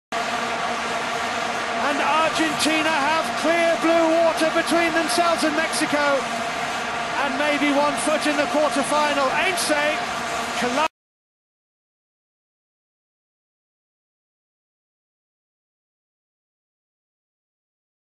Gabriel Heinze Hits Camera v Mexico
Gabriel Heinze hits head on camera and then punches it away.haha funny